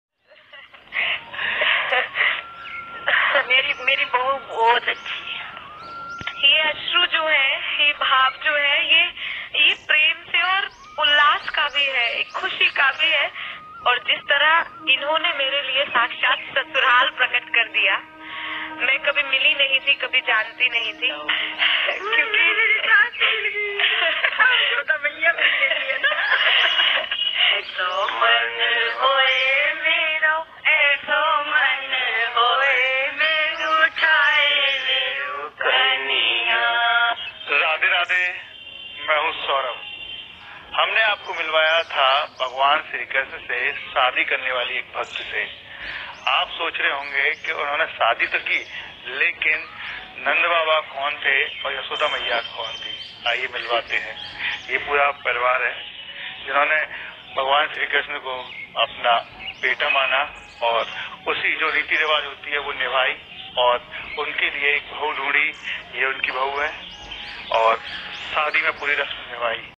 Vagwan se Shadi karne wali Priya ko Mila Sasural
Hey guys here I am sharing a video of A woman who married to Lord Krishna 🙏🙏✨✨Guys this is my first video on YouTube.